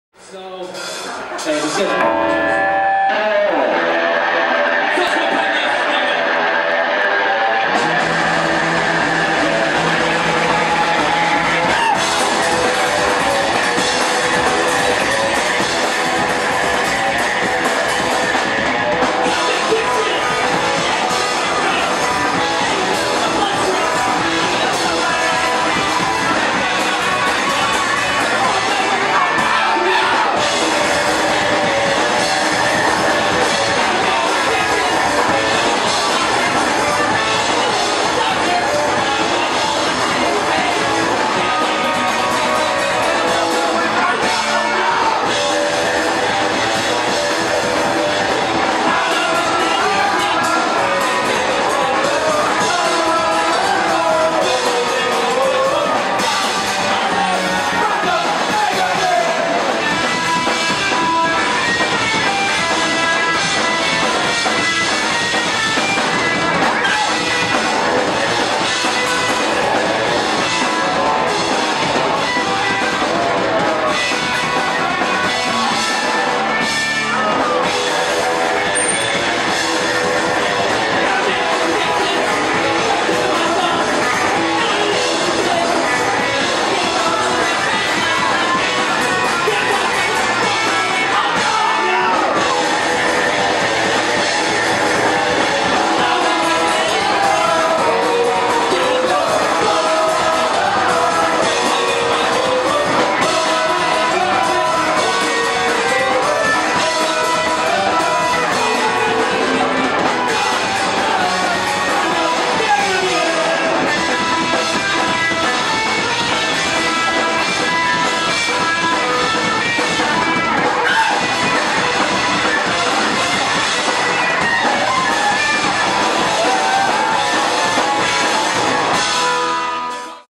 Night Birds 2012 live in Honolulu HI
Night Birds play 3 nights in Honolulu, HI for No Suck Fest 2k12.